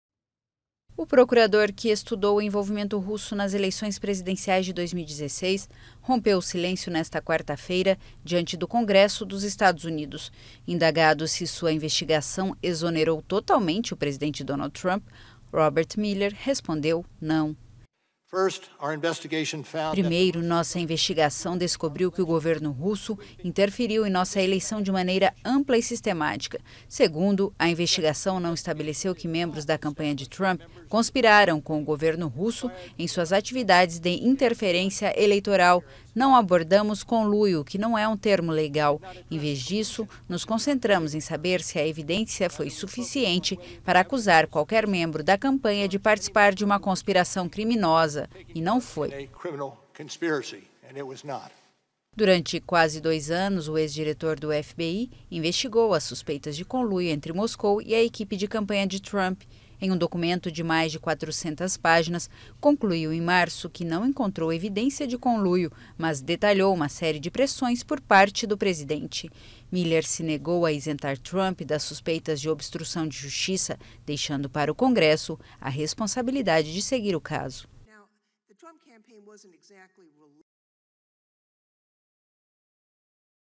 Investigações de Mueller não isentam Trump | AFP
O procurador especial que estudou o envolvimento russo nas eleições americanas, Robert Mueller, disse nesta quarta-feira em uma audiência ante o Congresso dos Estados Unidos que suas investigações não isentam o presidente Donald Trump.